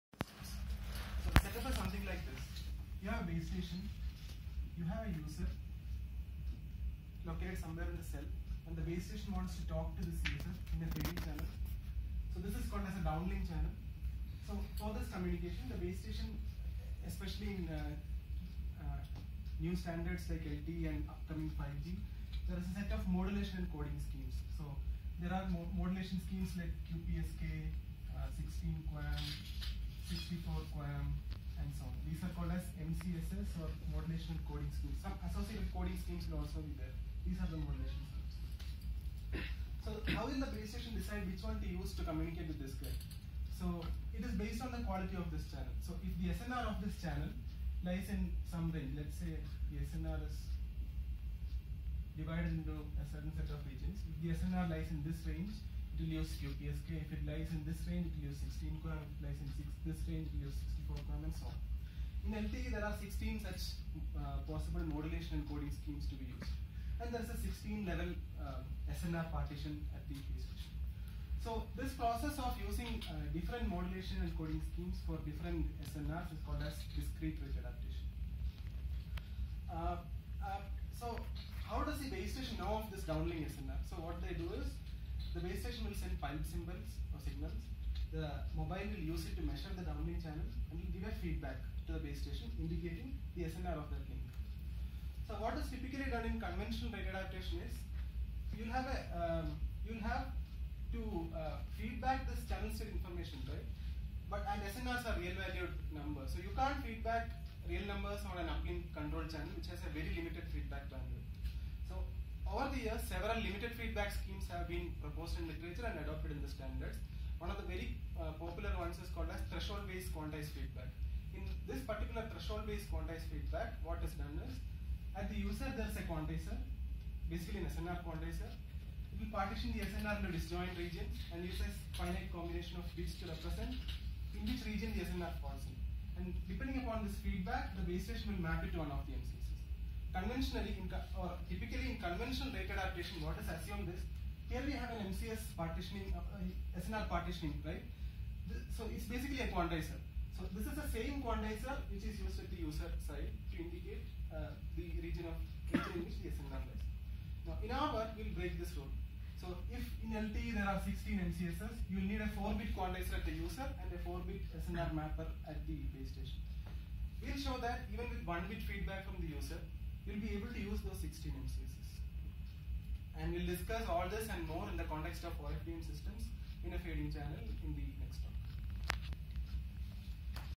3MT July 12, 2017 by Vineeth Kumar.
Throughput-optimal discrete rate adaptation for threshold-based feedback in OFDM systems.

Talk Scheduled on August 16, 2017 4-5PM at GJ Hall, ECE Dept.